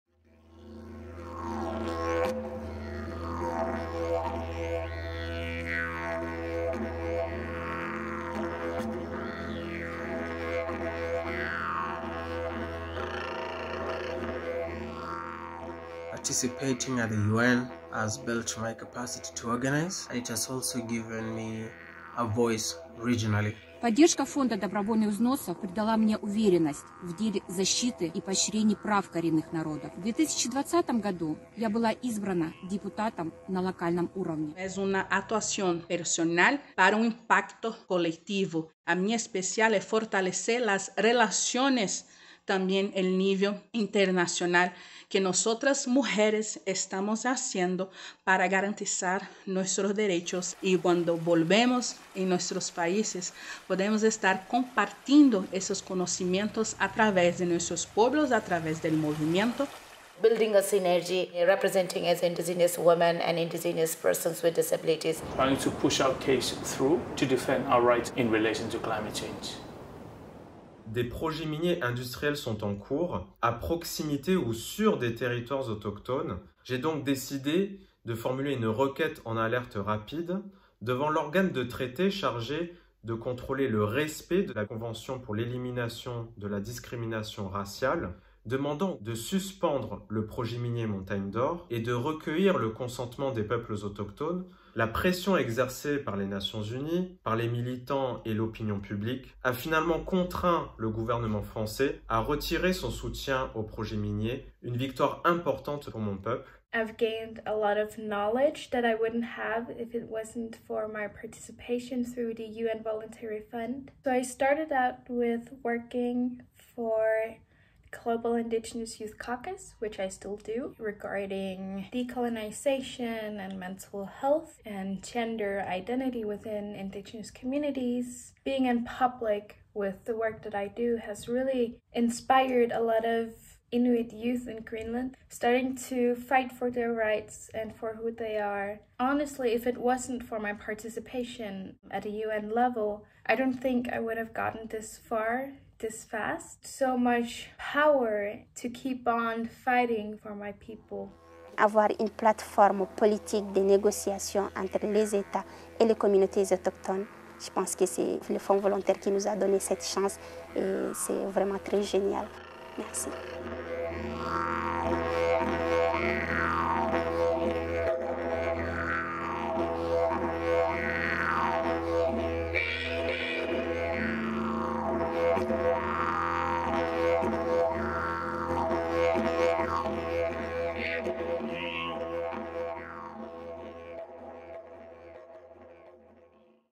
UN Voluntary Fund for Indigenous Peoples
A video featuring grantees of the UN Voluntary Fund for Indigenous Peoples, illustrating how the Fund is instrumental in supporting the rights of indigenous peoples across the different socio-cultural regions. Over 3'000 indigenous peoples from almost 100 countries have benefited from the Fund since it was established in 1985.